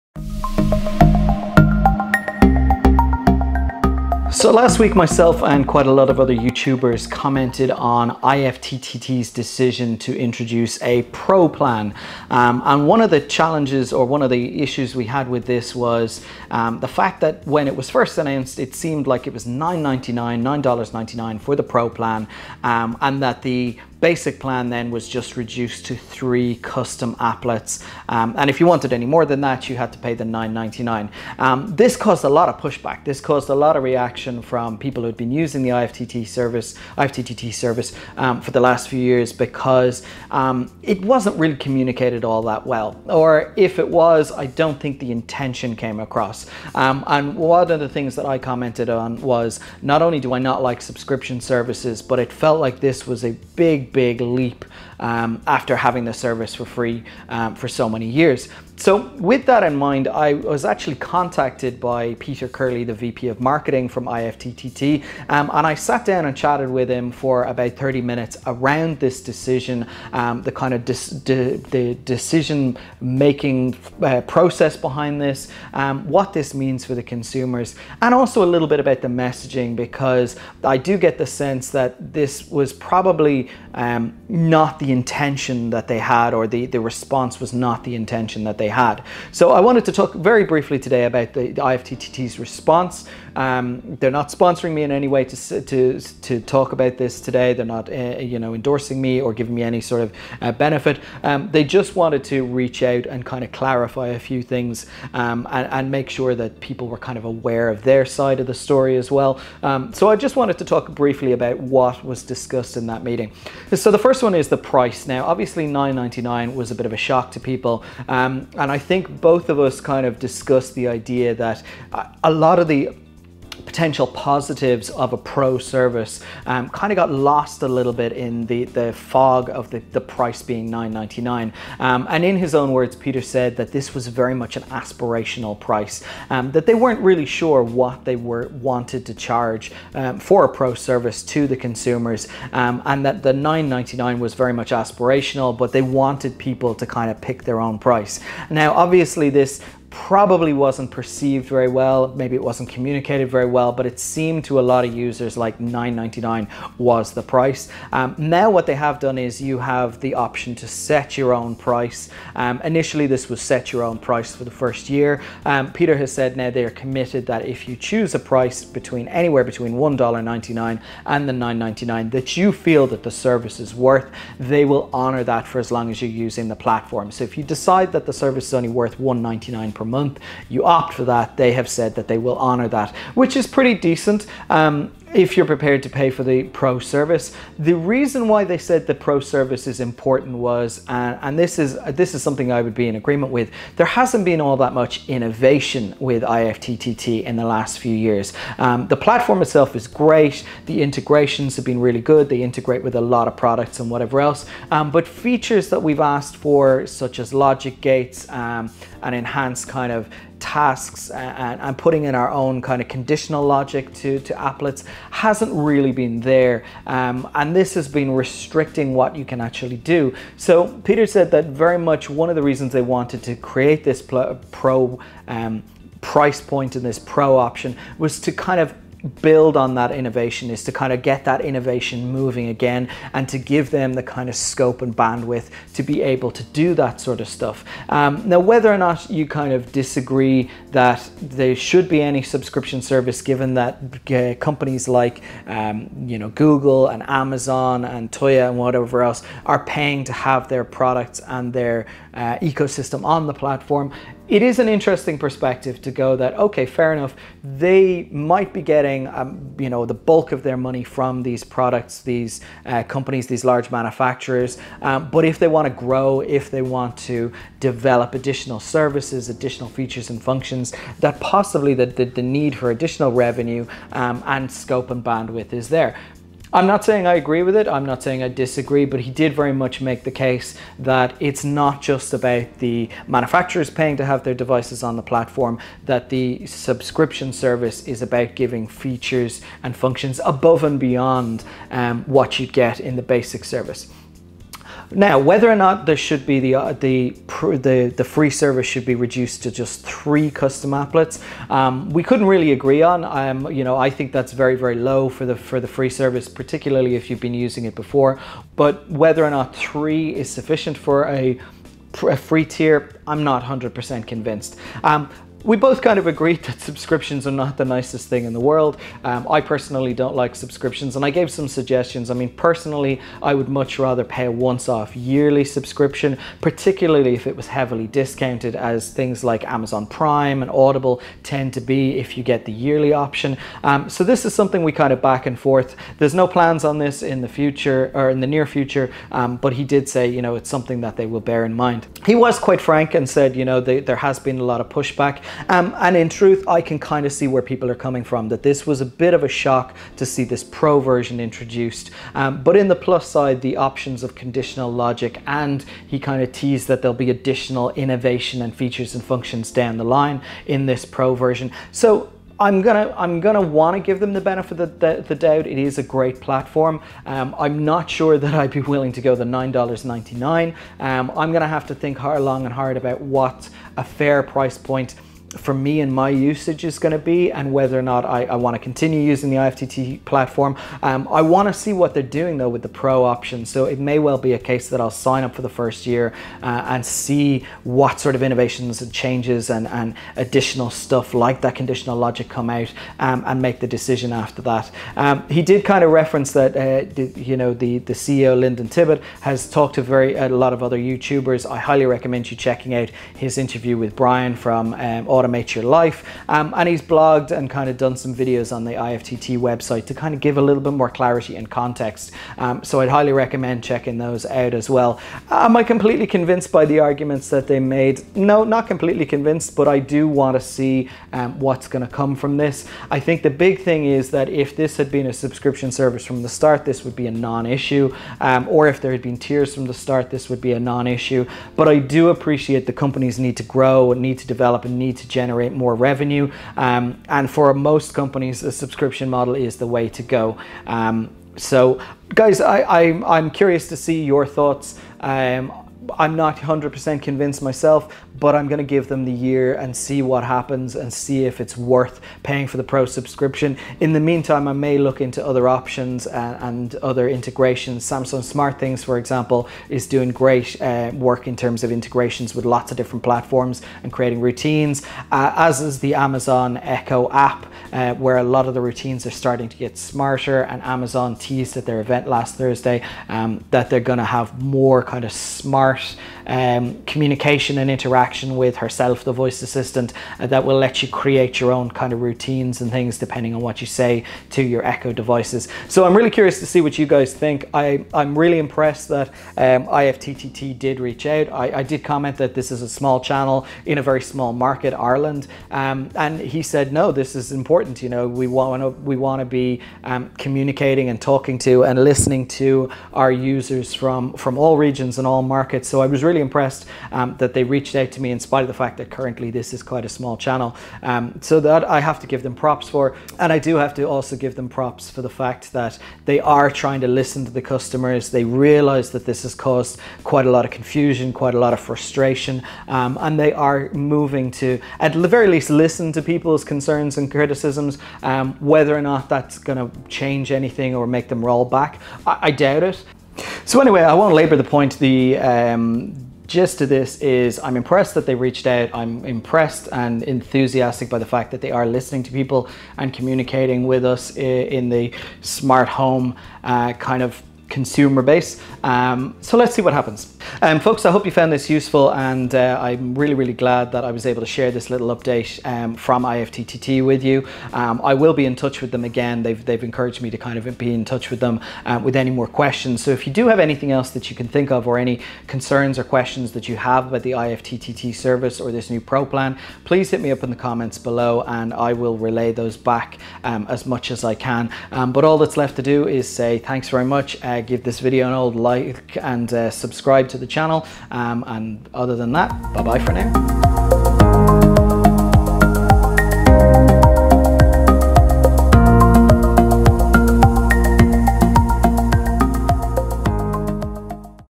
IFTTT Responded To My Video: Here's what we discussed
Last week I put out a video commenting on IFTTT's plans to create a Pro subscription service. There was lots of frustration online as well as confusion around the pricing and choose your own price model. At the time I expressed my surprise and concerns and shortly after I was contacted by Peter Curley, IFTTT's VP of Marketing.

We discussed the change and their logic behind changing to a subscription model and in this video I talk about some of the things we discussed as well as my thoughts following the conversation. While I'm still not sold on their decision we tried to come to an understanding about the context and need for this change.

I'd highly recommend checking out this interview by Brian from Automate Your Life with Linden Tibbets, the CEO of IFTTT